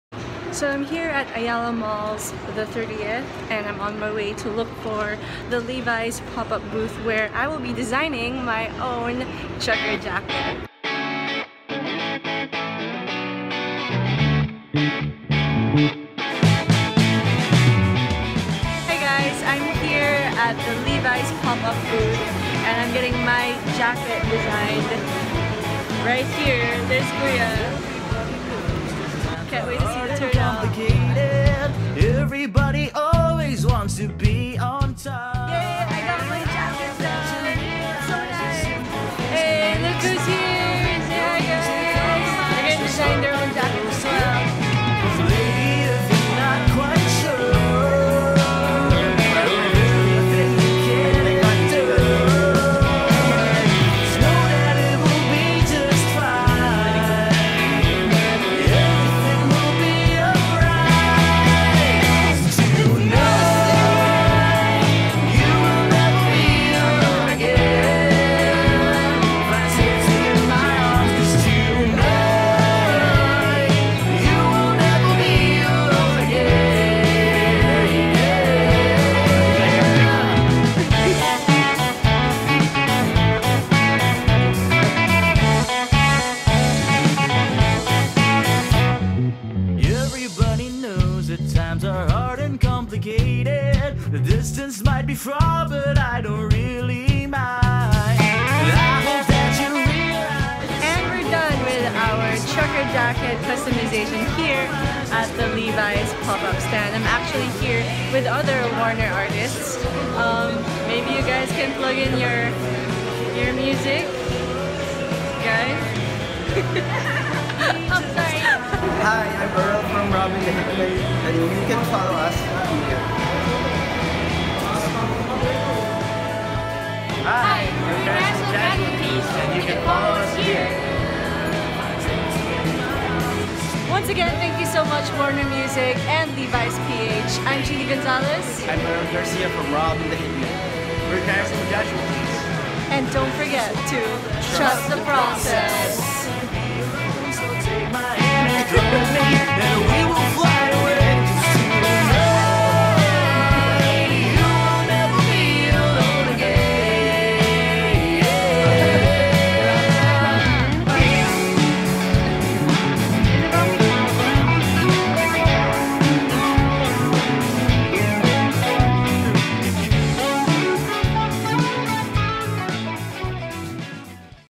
Levi's Vlog: Trucker Customization with Cheenee Gonzalez, Carousel Casualties, and Rob & the Hitmen)
Thank you so much to Warner Music Philippines and Levi's Ph for inviting us to customize our own trucker jackets!

or text her manager at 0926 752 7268